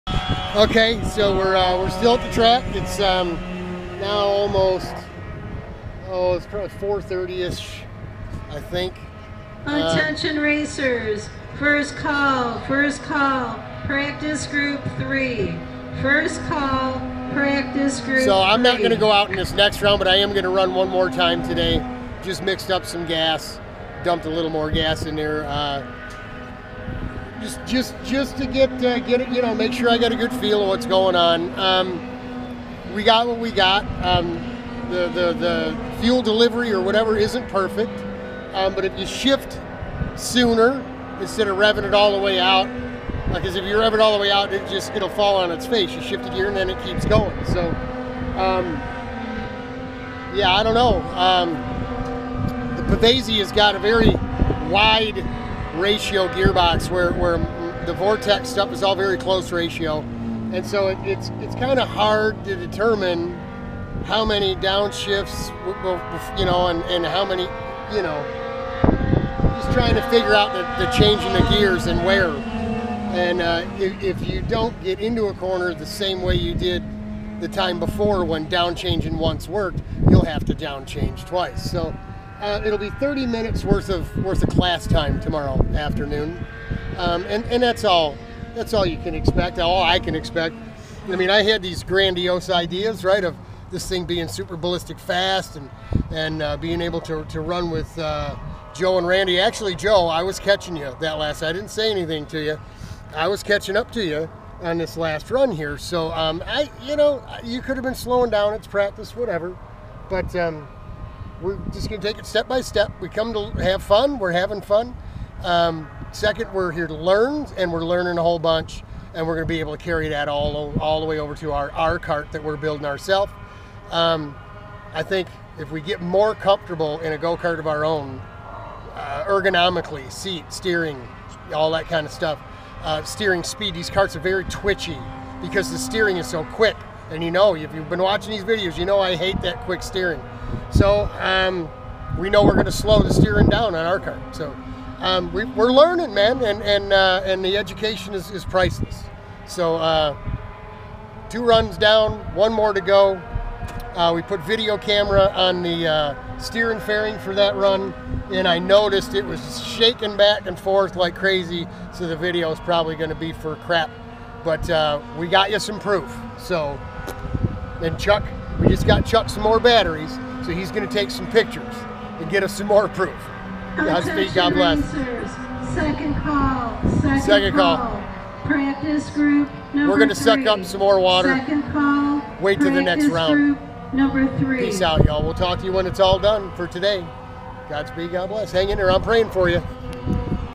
Friday Practice - Post Run 3
Run 3 was just 2-3 laps counting the out lap. The kart was low on fuel and I didn't have time to add more as our group was being called. I was making good progress, my gear selection was much better and kept my momentum up entering and my center/off corner speed so everyone wasn't pulling away off every corner. I could have done 2 more runs but decided to get our area cleaned up and pack away the Anderson bodywork molds we picked up. I would add fuel to the kart and get it warmed up for the last run of the day at about 5:30pm. THAT run you wont see as I forgot to turn on the camera! Run 4 was MUCH - MUCH Better! I was getting after it, going as deep as anyone else and had just the 2 corners at each end of the track that needed more attention. At the end of the day I was confident with repetition I could race with some guys and learn a bunch more with a race distance of 25 minutes.